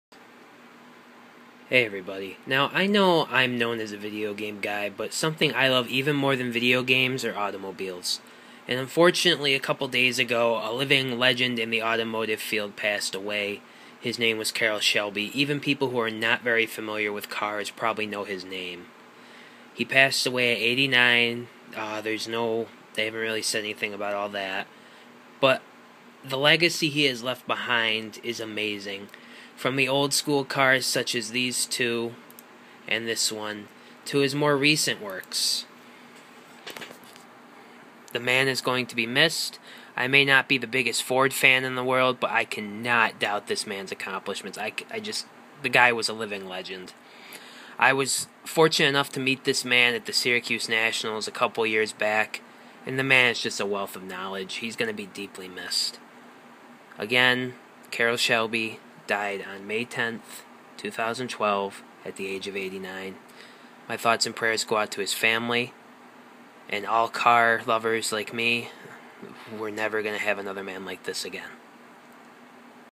Carol Shelby dead at 89
Deeply saddened by this. This man was my role model. My thoughts and prayers go out to his family at this difficult time.

The world has lost one of it's best human beings.